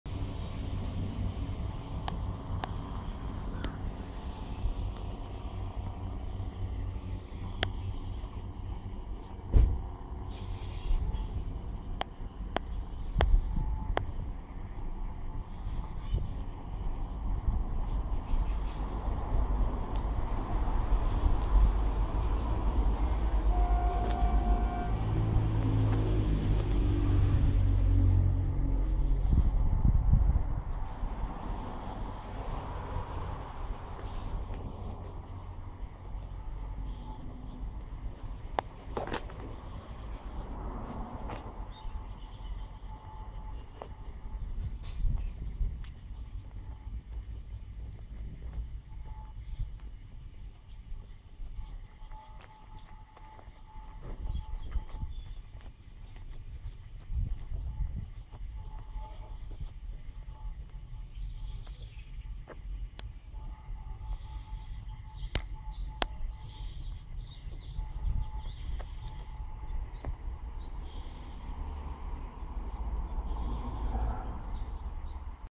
Delamere Train Station
Train Service From Manchester Piccadilly To Chester Via Altrincham Calling At Delamere